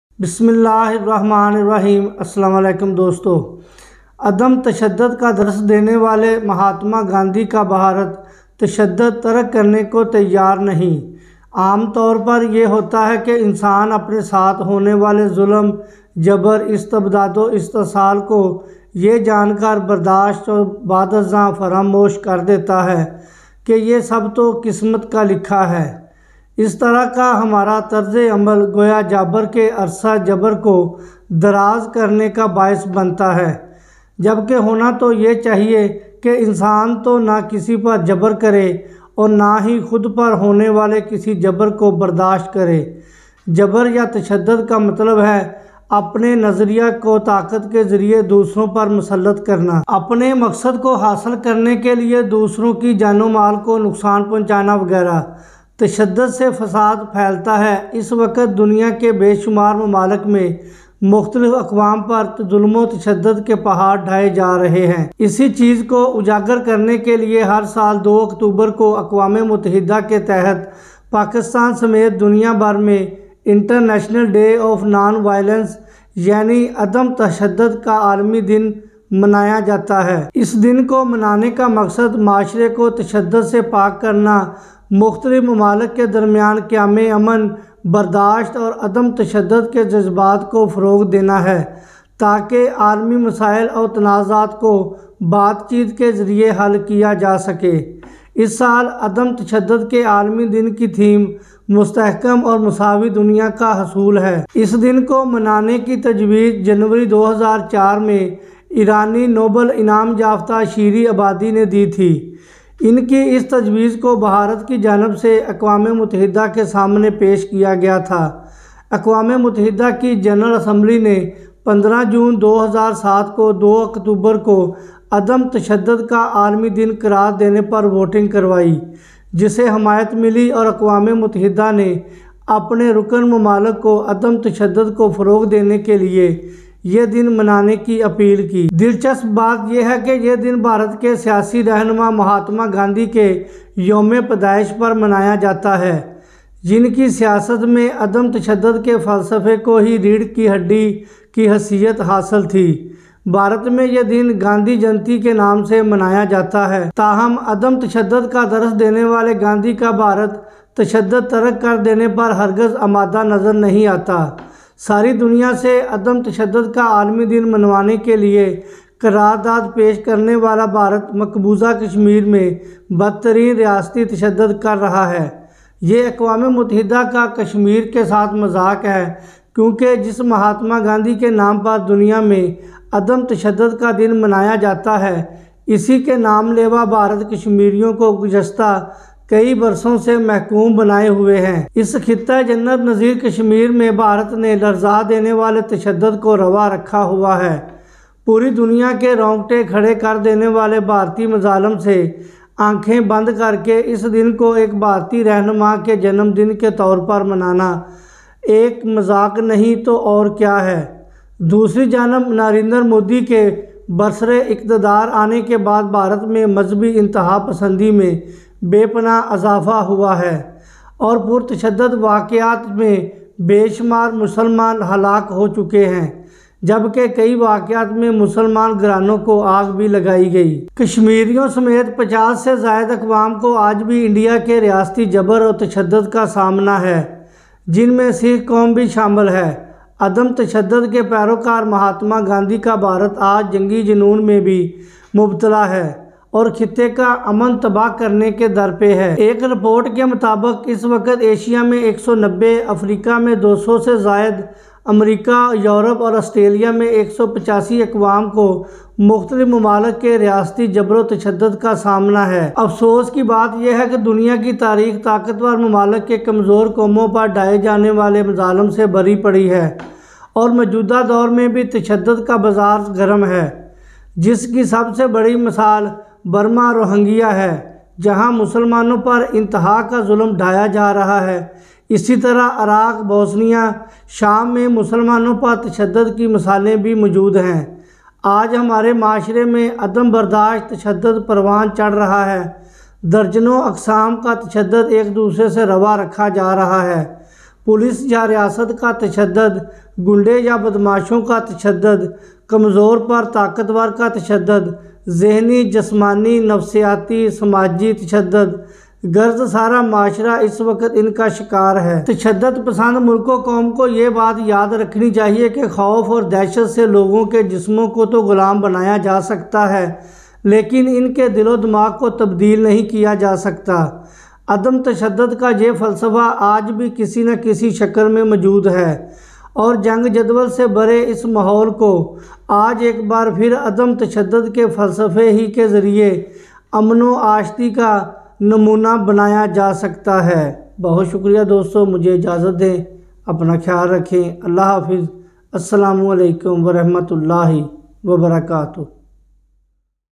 World Day of Non-Violence, the market for violence in the world is still hot || Asif Live
The day was proposed by Iranian Nobel laureate Shirin Ebadi in January 2004. His proposal was put forward by India to the United Nations. The United Nations General Assembly voted on June 15, 2007 to declare October 2 as the International Day of Non-Violence, which was supported, and the United Nations called on its member states to observe the day to promote non-violence.